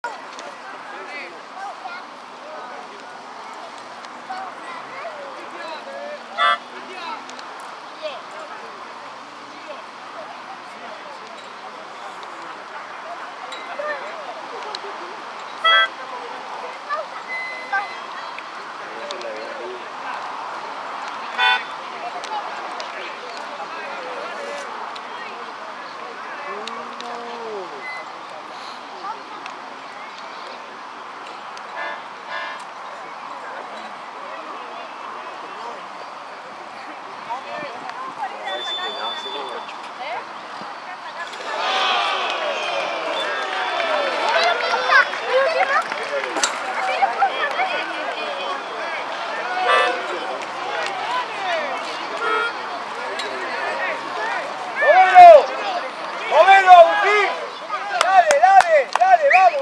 Camilo Ruffa para la 9na de Arsenal (Liga) ante River Plate. 30-4-2023.
Video. Cristian Rompani.